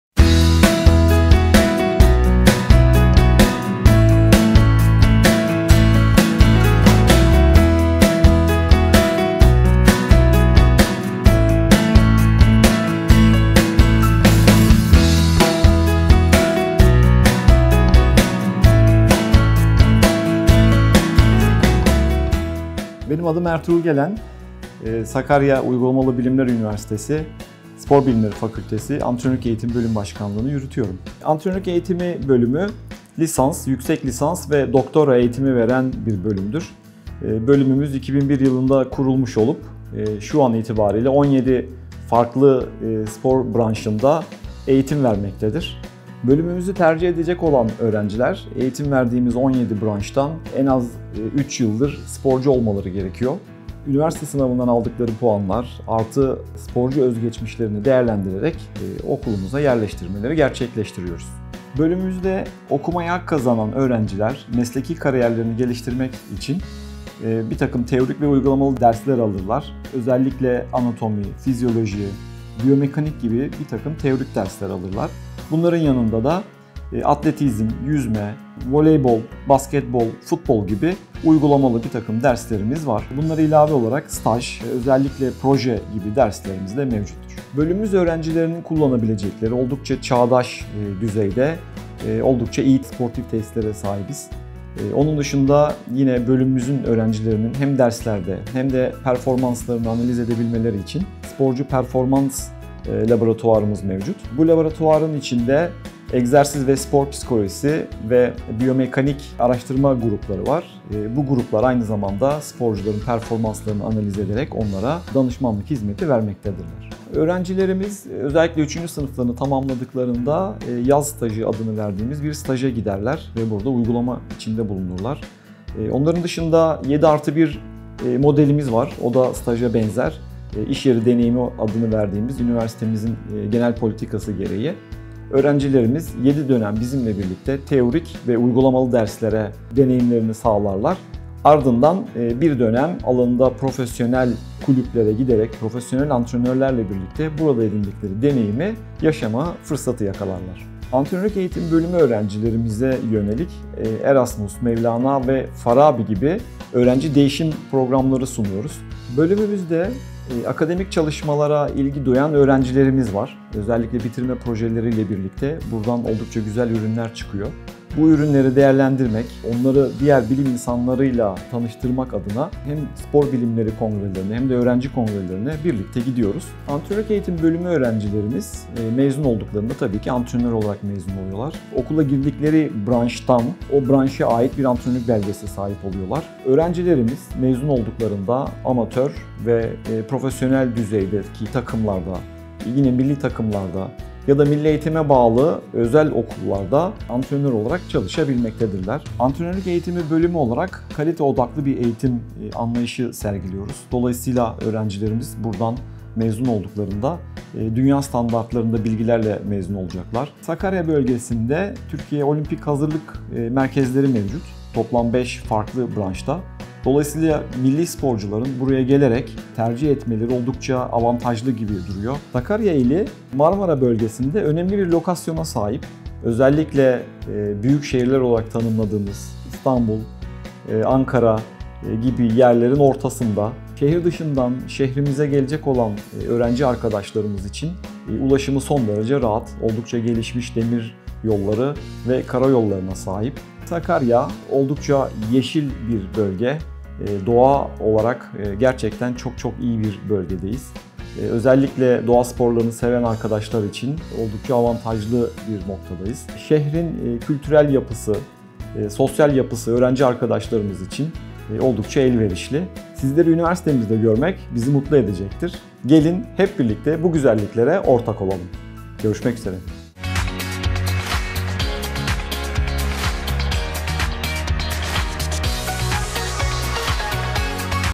Antrenörlük Eğitimi Bölümü
Spor Bilimleri Fakültesi Antrenörlük Eğitimi Bölümü Başkanı Prof. Dr. Ertuğrul Gelen, bölümü anlatıyor.